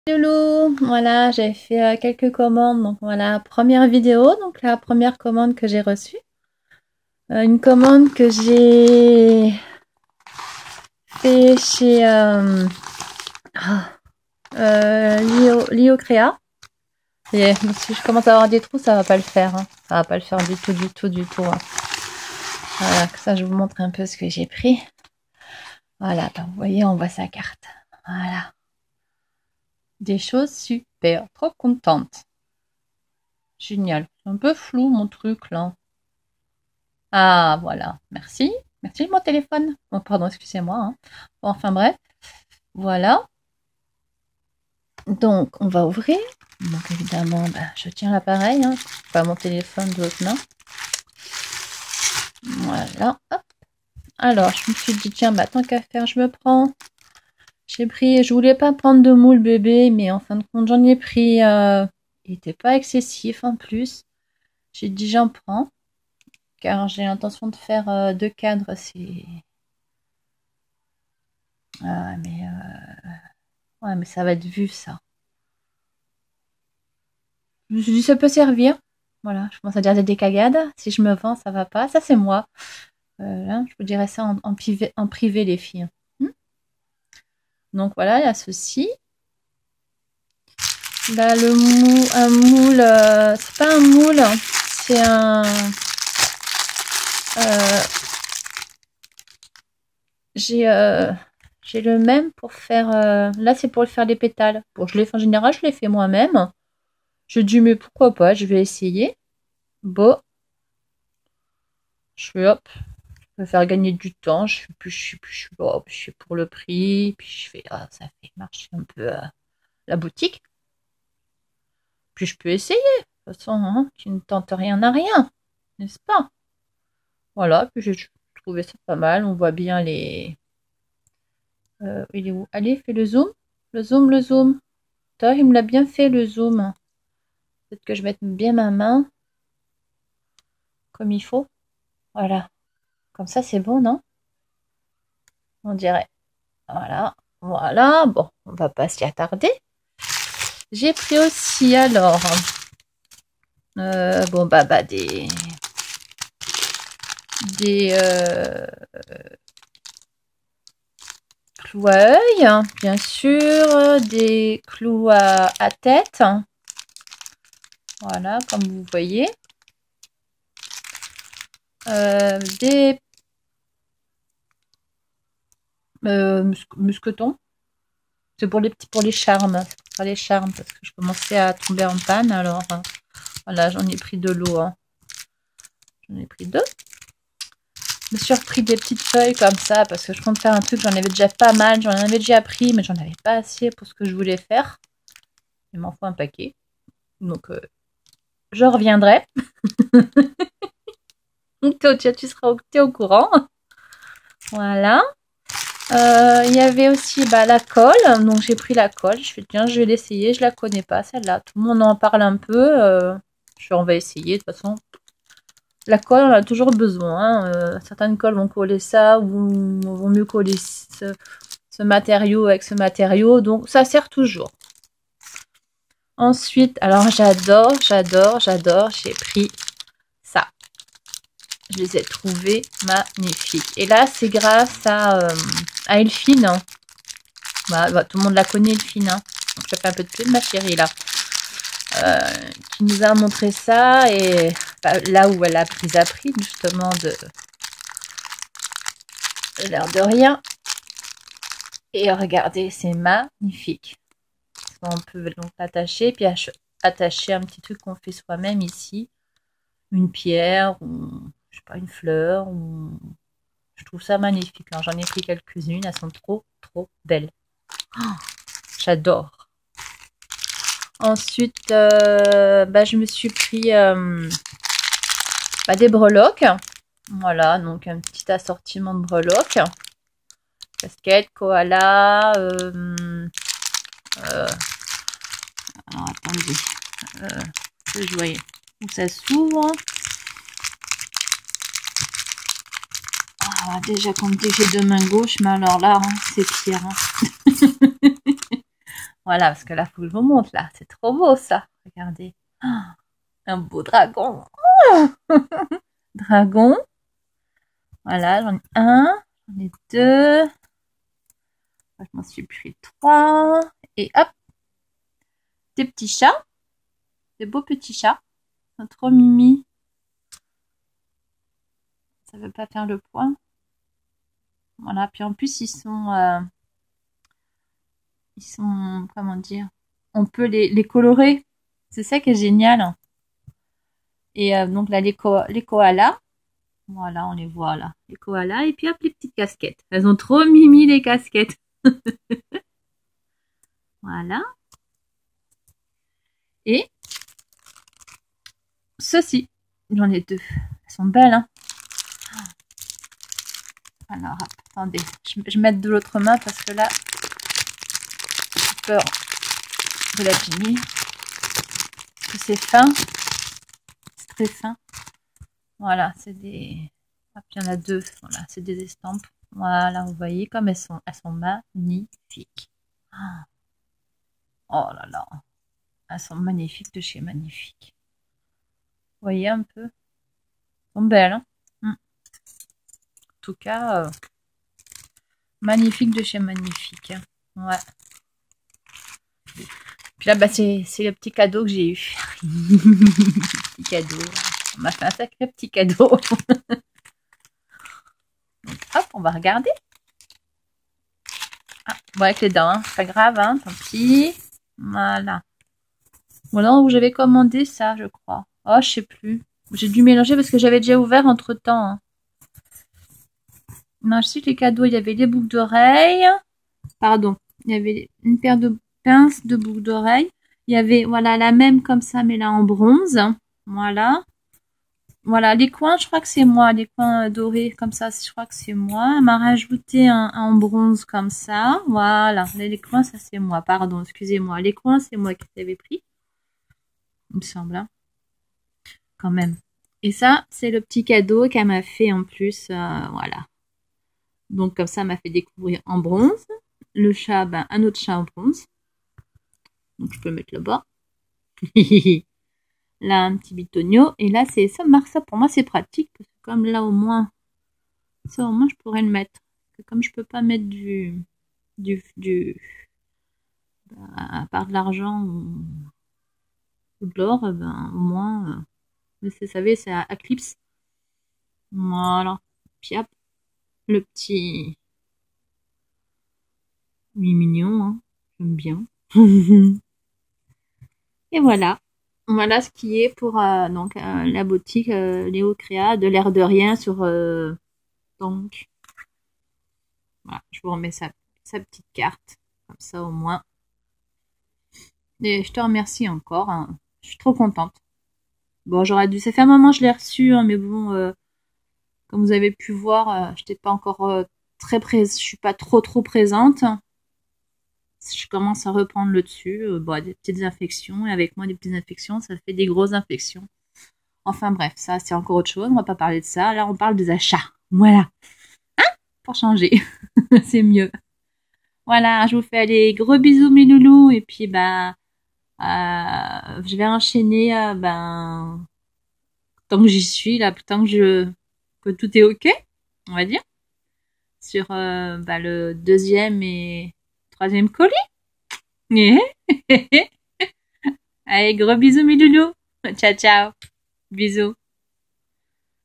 Achats de la boutique L'air de rien !
Et dsl pour les achats " boutique " passee ! Pour les prochaines videos 😉
C'est ma premiere !!! J'etait pas tres chaude mais pourquoi pas ( video achats )
Gros bisous mes Loulous !😘